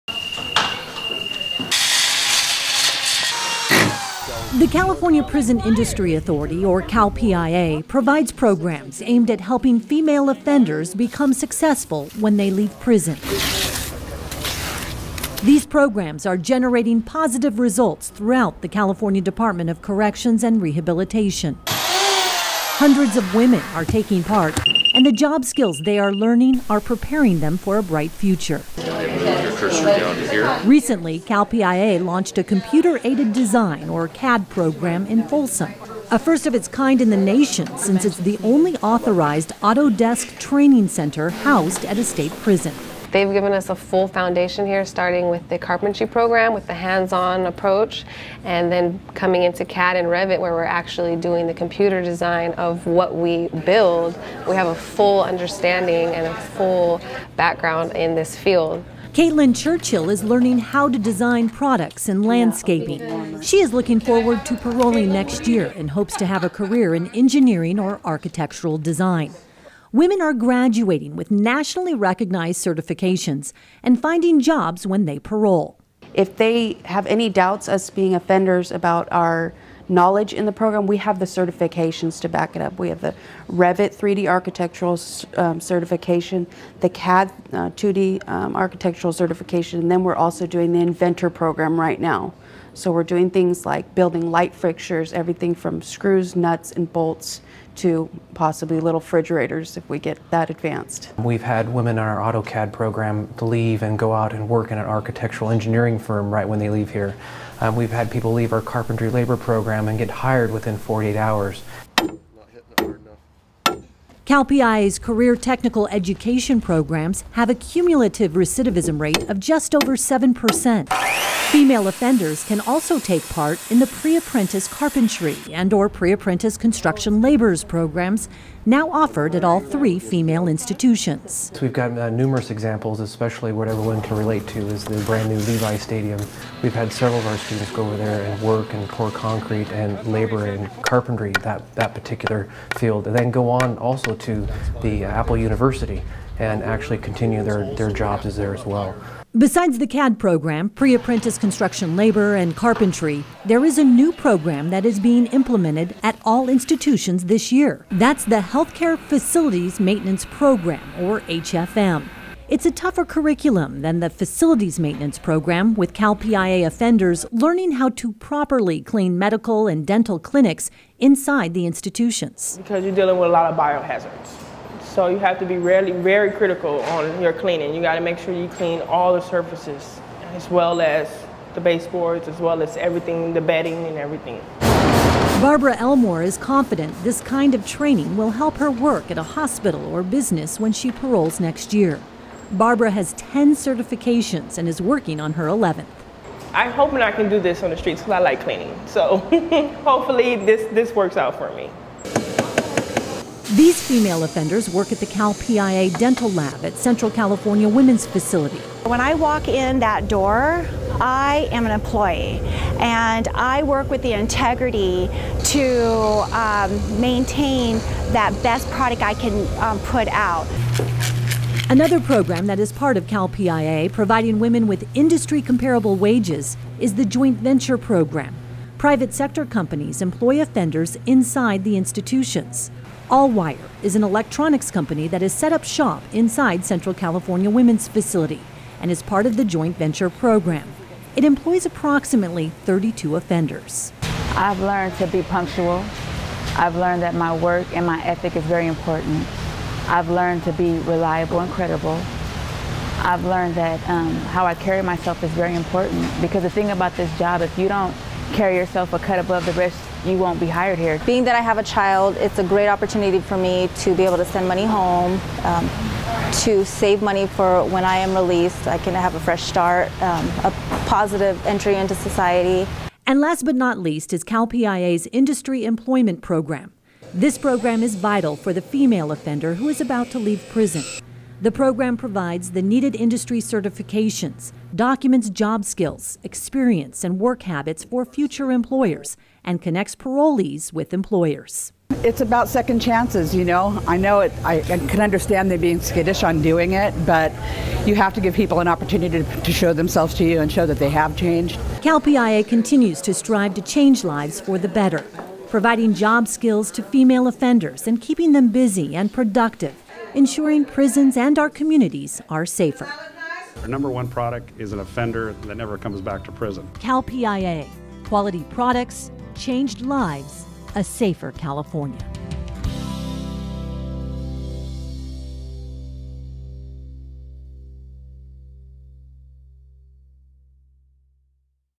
CALPIA Female Offender Programs
California Prison Industry Authority provides valuable job skills and training to women at all three female institutions in the state.